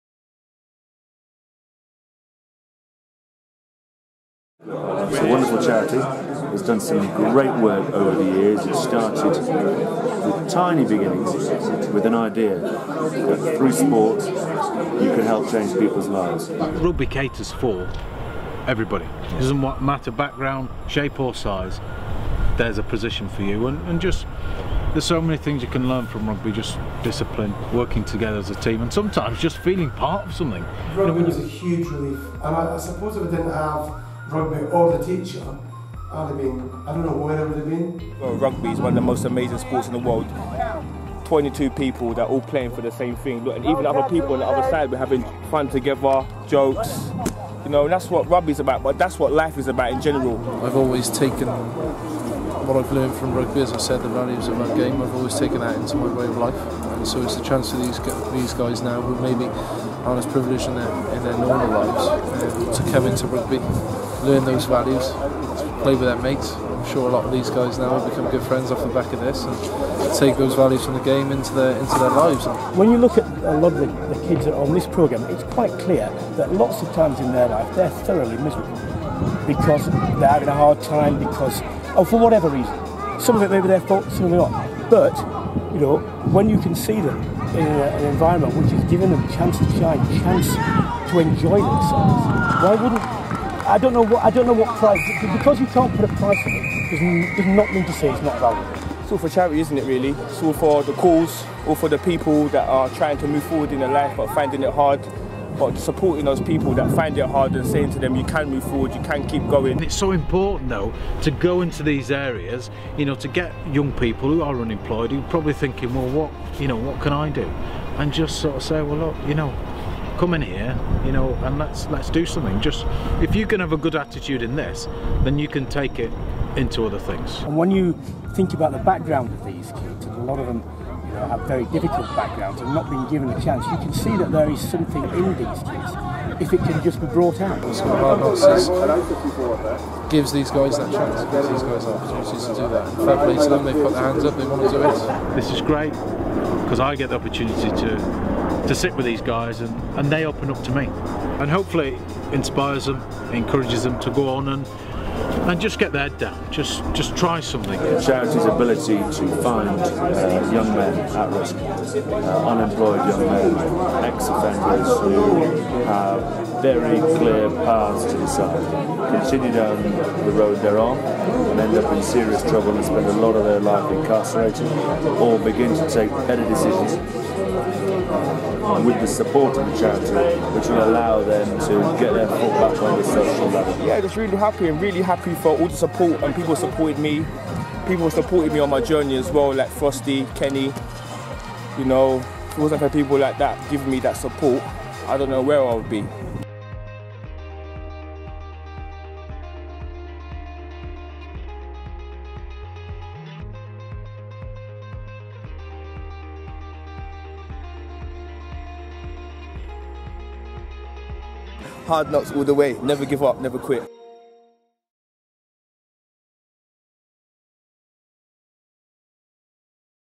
Rugby Matters by The School of Hard Knocks
To coincide with Rugby World Cup 2015 we're focussing on the power of rugby at the grassroots level to change lives and create futures with our RugbyMatters campaign. We asked several famous players to tell us why RugbyMatters to them in this video. We now want you to tell us why RugbyMatters to you. Tell us your stories in the comments and send us your pictures and videos to @sohkcharity on Twitter